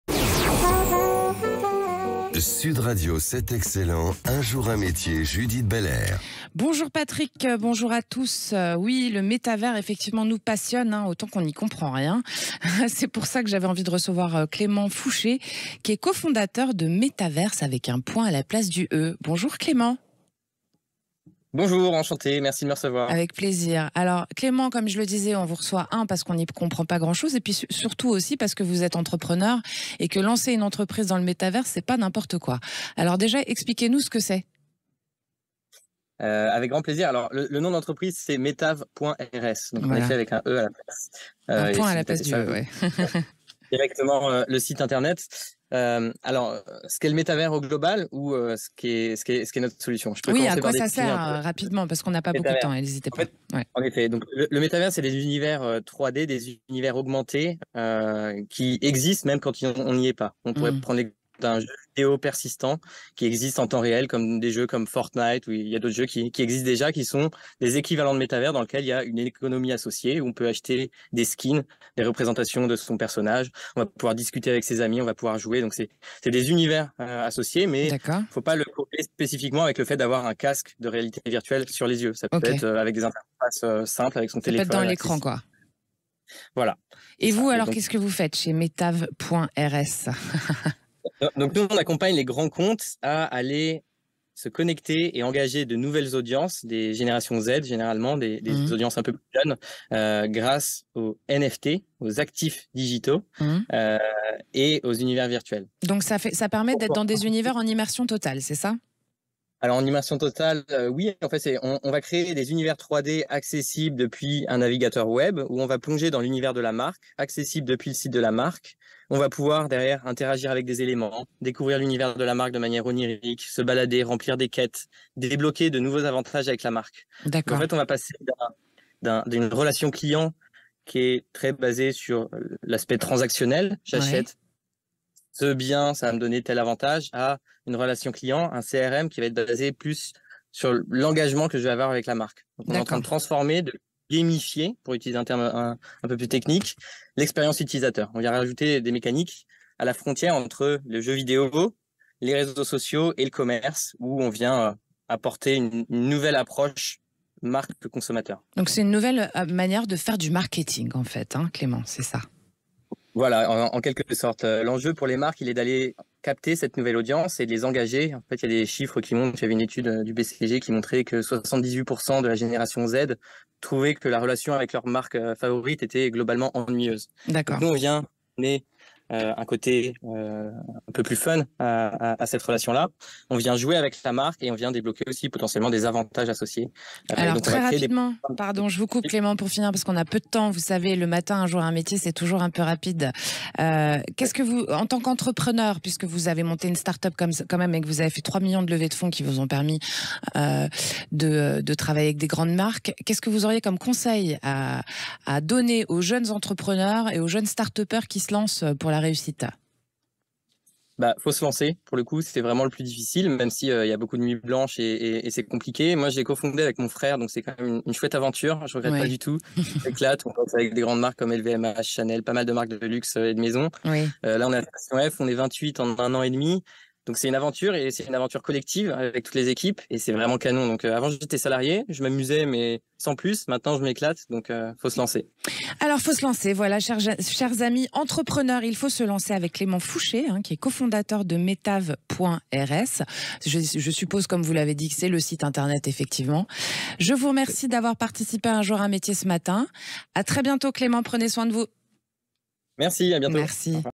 Le Métavers, c'est quoi ? Rencontre avec Clément Foucher, cofondateur de METAV.RS
Un jour, un métier avec Judith Beller, tous les vendredis à 6h30 !

Son invité est Clément Foucher, cofondateur de METAV.RS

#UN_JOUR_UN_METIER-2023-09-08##